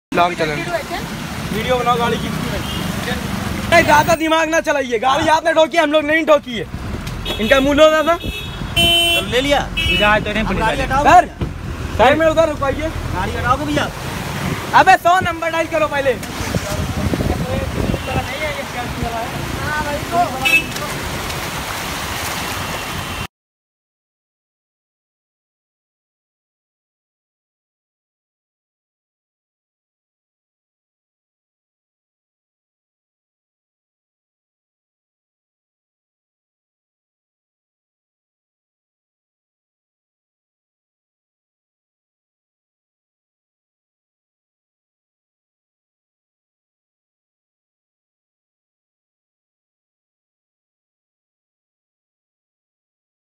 OLA DRIVER EXPOSED
This accident happen in lucknow a taxi in full speed crash into fascino from behind the driver is so careless he just trying to run away but we stopped him  UP 32 HN 6629